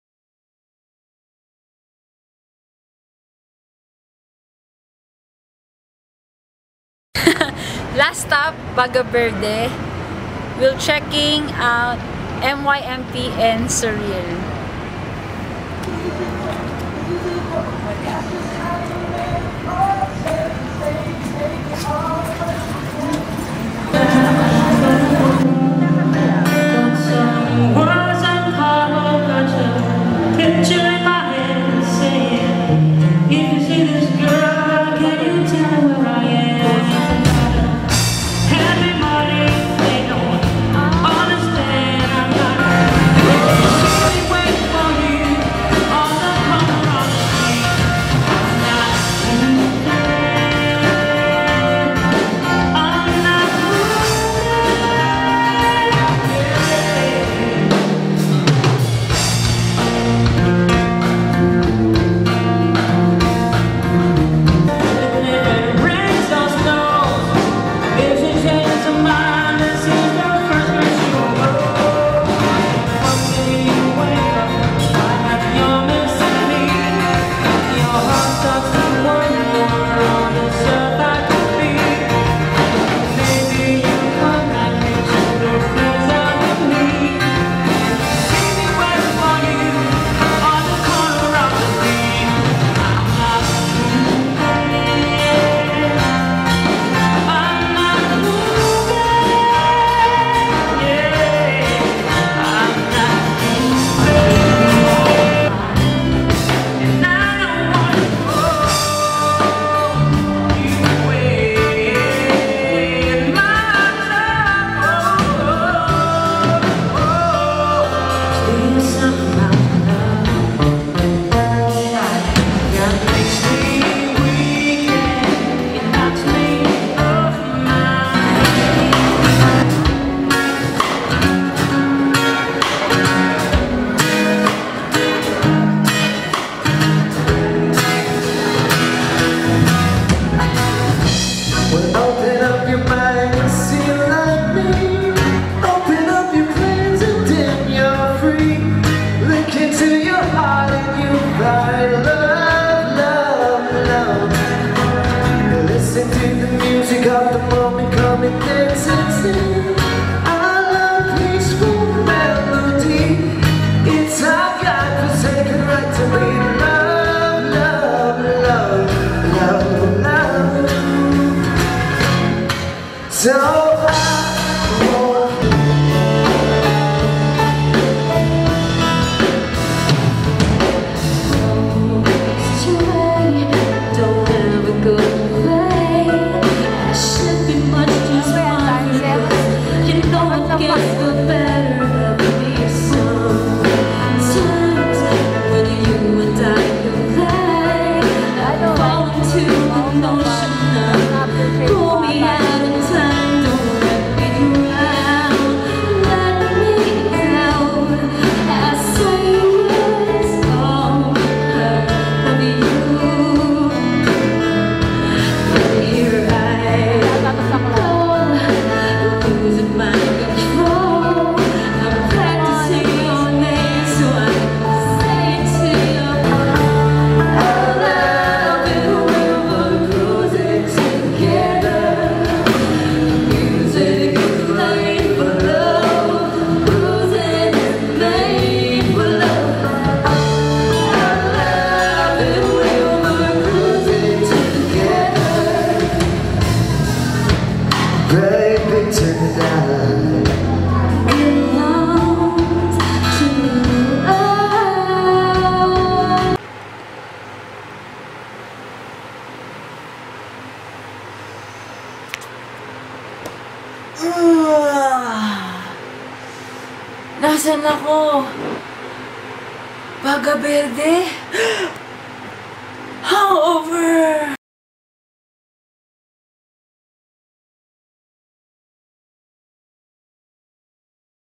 SURREAL @ Bagaberde, Roxas Blvd.
an excerpt from Surreal's set at Bagaberde... thanks to mishalesexylove for the video...